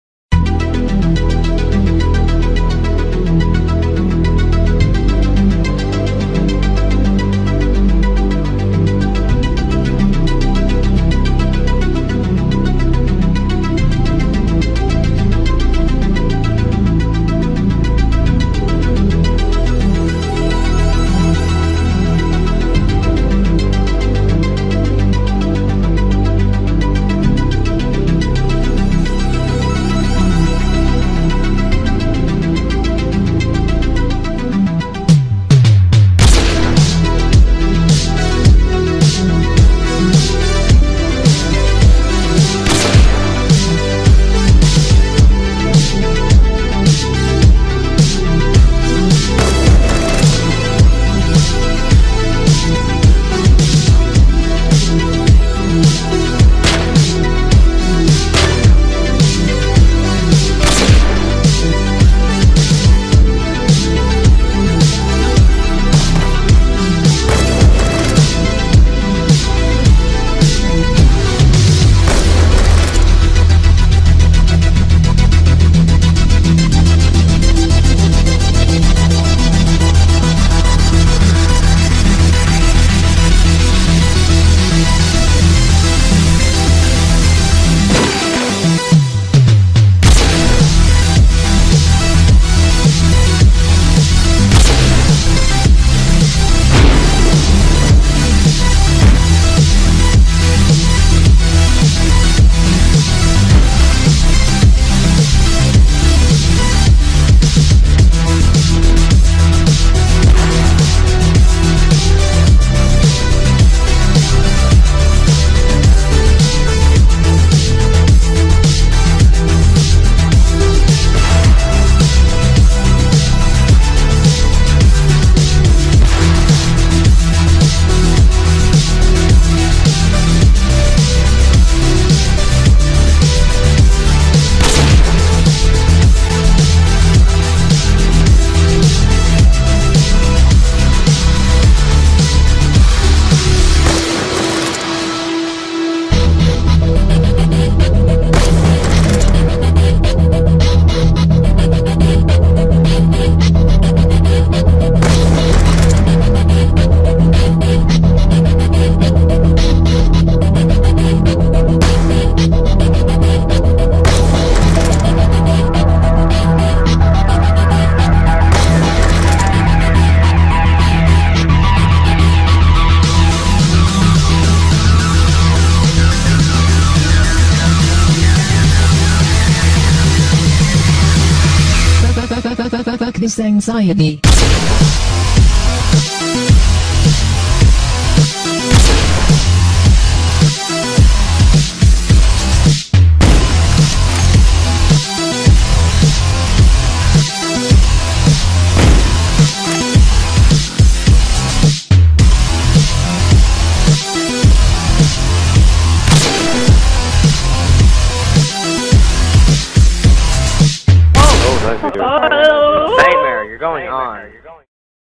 United Dualtage | By Render | 100th Upload!
This is a dualtage i edited for some friends in the Ozar sniping clan.
The Players--Ozar Nightmare and Ozar Sky.
This is also my 100th video!!